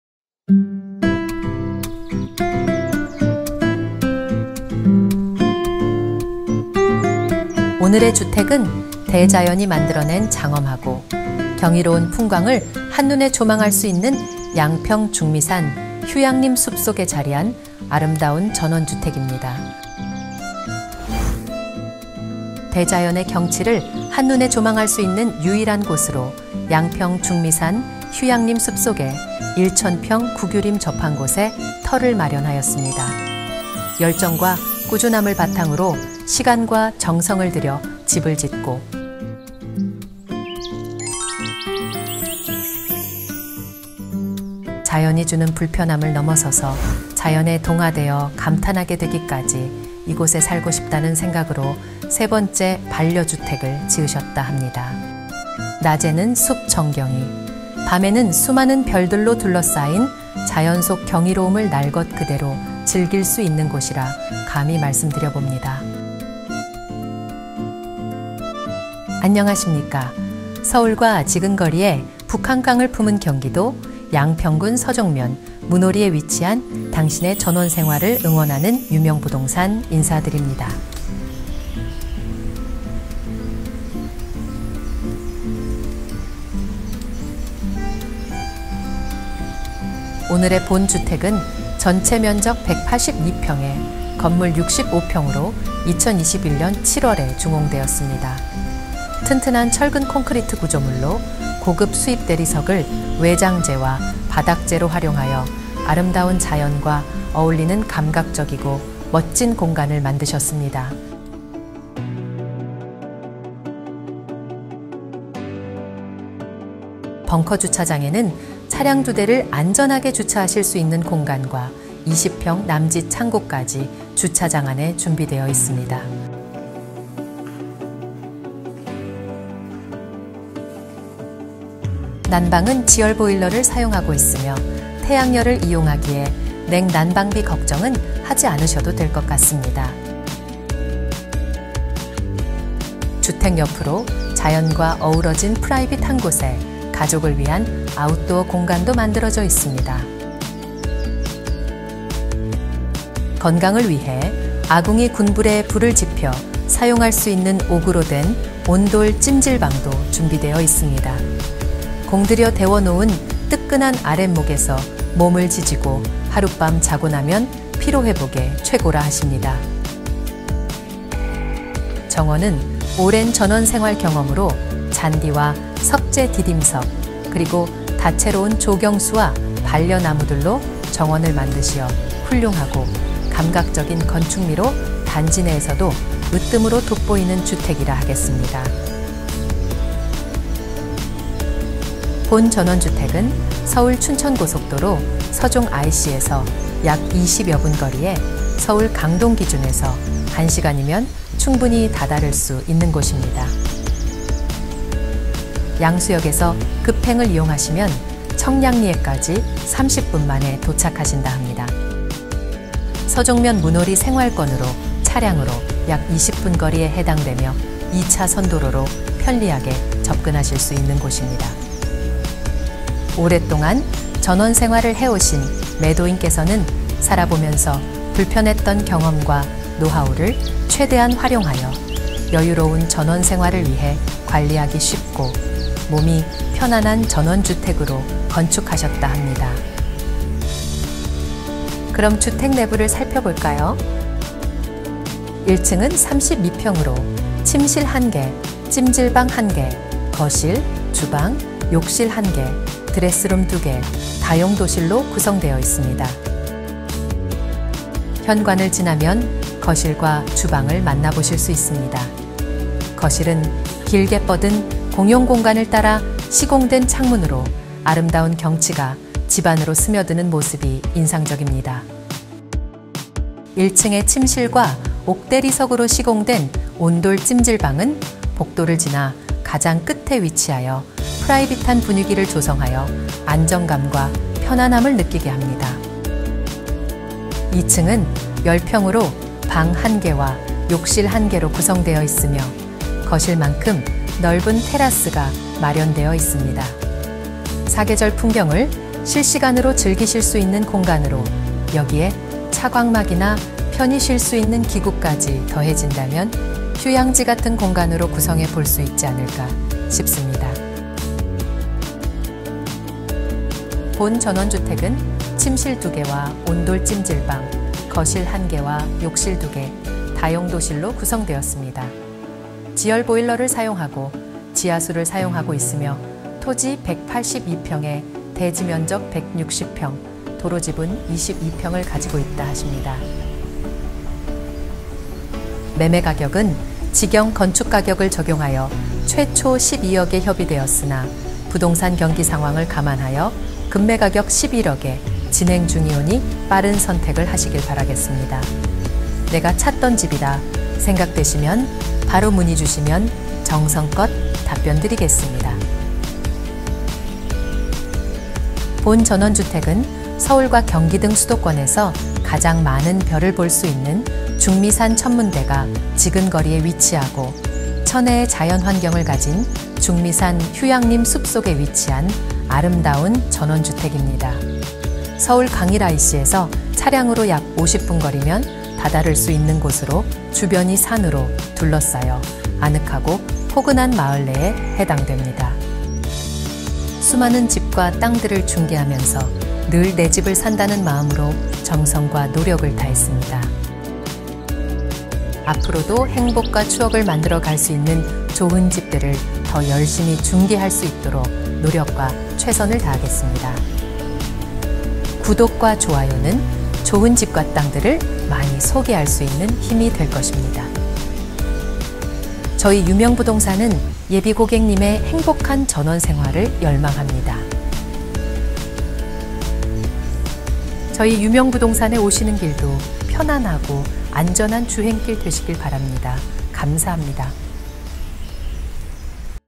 원가무시 가격할인! 양평 고급전원주택 급매물 매매 ,강일50분🏡양평부동산 급매물 매매❮유명부동산NO.3987❯중미산휴양림, 고급전원주택
위치 :  경기 양평군 옥천면 / 대지 602.42㎡ (182평) / 건물 291.28㎡ (65평) / 철근콘크리트구조
매매가 9억9천만원 (최초가격 12억에서 2억1천인하) ☞ 바로가기
[더 많은매물보기]

[HOUSE PLAN]
대지위치 : 경기 양평군 옥천면 신복리 (중미산휴양림 인근)
대지면적 : 대지 602.42㎡ (182평)
건물면적 : 건물 291.28㎡ (65평)
건물규모 : 지상2층
준공년도 : 2021년 7월 16일
주차대수 : 지하주차장 2대
공법 : 철근콘크리트주택
조경상태 : 마당전체 대리석시공, 조경수, 합성데크
기타 : 지열보일러,태양열/마을상수도/오폐수직관/시스템에어콘, 전기지중화
급매가 : 9억9천만원 (최초가격 12억에서 2억1천인하)

중개사무소 : 유명공인중개사사무소
사무소위치 : 경기도 양평군 서종면 문호리 352-1번지, 재원빌딩 1층
[홈페이지 바로가기]

● 네이버, 다음 검색창에 양평유명공인중개사를 검색하시면 양평군의 최다급매물 정보를 확인하실 수 있습니다.

감사합니다.